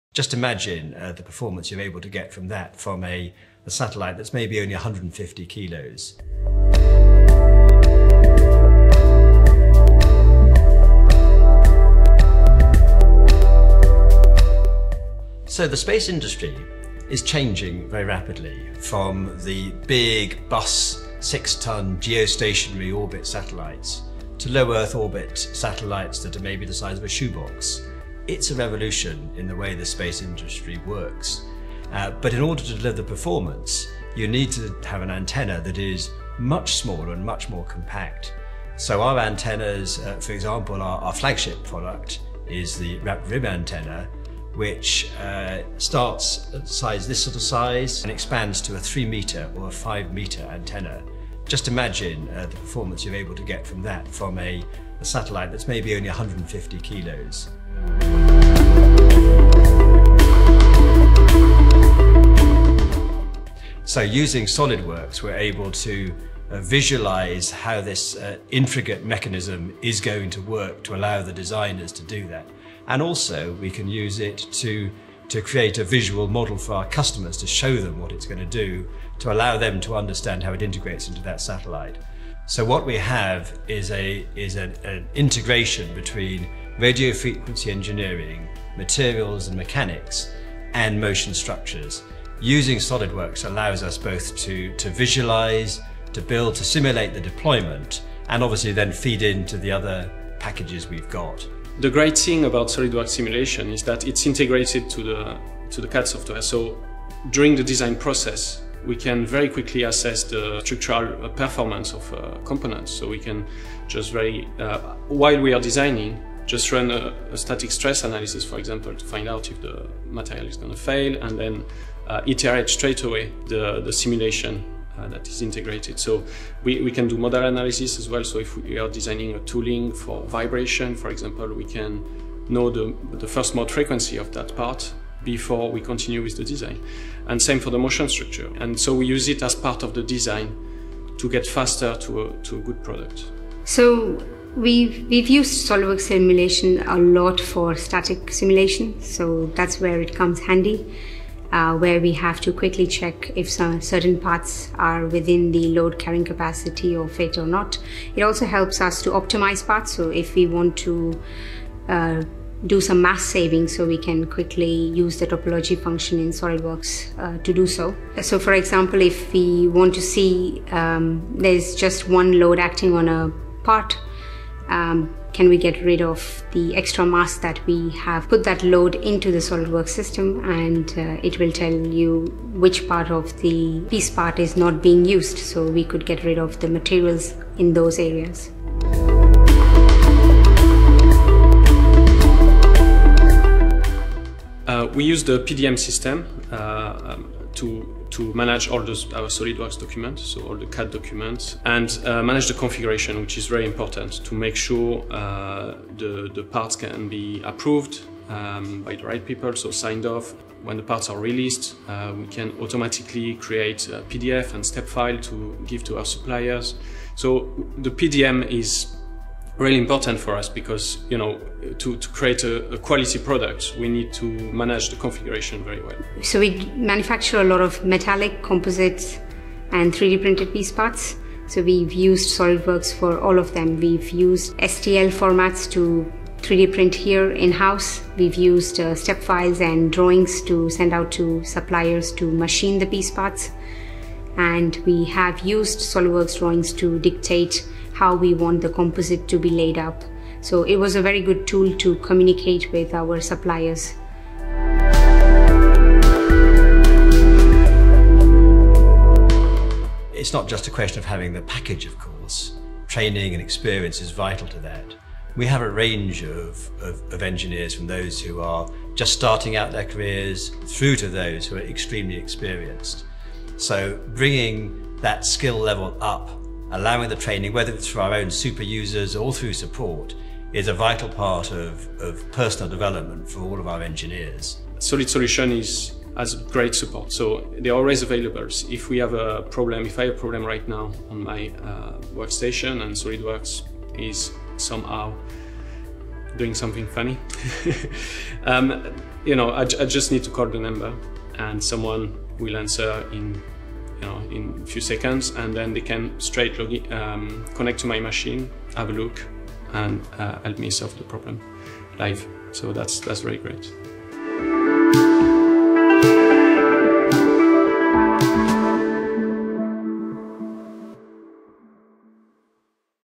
SOLIDWORKS in Outer Space! | Oxford Space Systems
Industries are undergoing a state of transformation and disruption across all sectors, and the space industry is no exception. UK-based start-up Oxford Space Systems is paving the way for new satellite and antenna technology innovations to drive a revolution throughout the industry.

“The space industry is changing very rapidly from the big bus 6-tonne geostationary orbit satellites to low Earth orbit. Satellites that are maybe the size of a shoebox. It's a revolution in the way the space industry works.”  - Sean Sutcliffe, Co-Founder and CEO at Oxford Space Systems.

In order to validate their concept and begin to consider manufacture and production, Oxford Space Systems began using SOLIDWORKS for conceptual designs, Visualize for modelling, and Simulation to analyse the design in its required conditions, along with PDM for secure, streamlined CAD data management.

"Using SOLIDWORKS allows us to design, build and simulate the deployment of our products." – States Sean.

For Vincent however, using SOLIDWORKS was a no brainer when it came to choosing a solution that is technically capable of delivering high-end CAD designs as well as seamless integration and communication with various suppliers.

"It's [SOLIDWORKS] is our software of choice as it's affordable and widely used throughout the industry, supporting with compatibility. - Functionality that comes with SOLIDWORKS, the compatibility with other software and the supporting tools such as the PDM." - Vincent Fraux, Co-Founder and Technical Specialist at Oxford Space Systems.

Discover how the Solid Solutions Entrepreneur Programme could help your business get off the ground